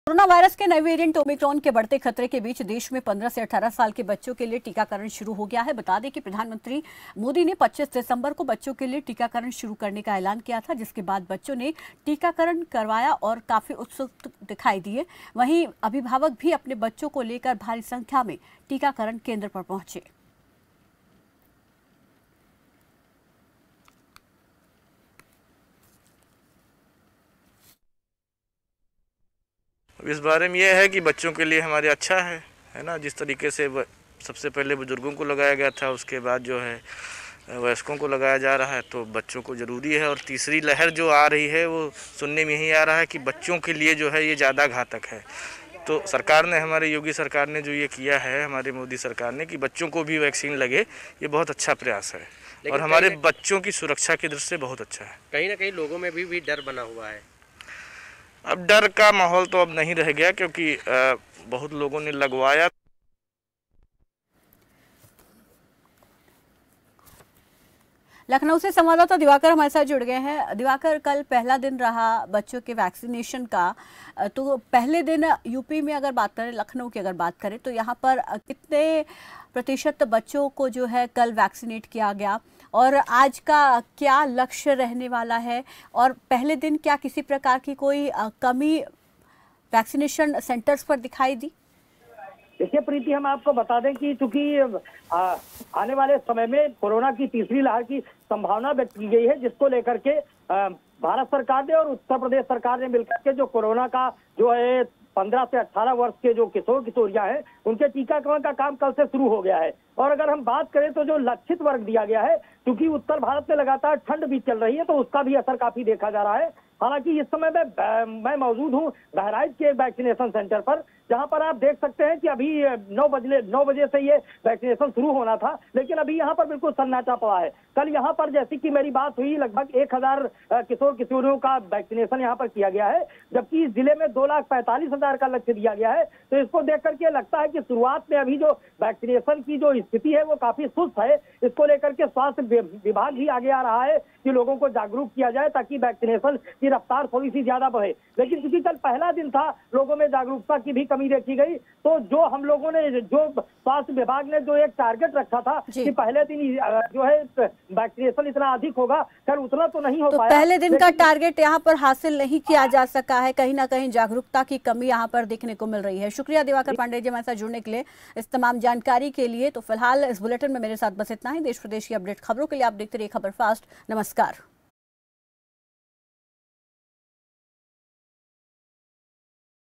UP: Omicron का मुकाबला Vaccine से...टीकाकरण को लेकर उत्साहित दिखे बच्चे
टीकाकरण को लेकर उत्साहित दिखे बच्चे
15-18 साल के बच्चों ने कराया टीकाकरण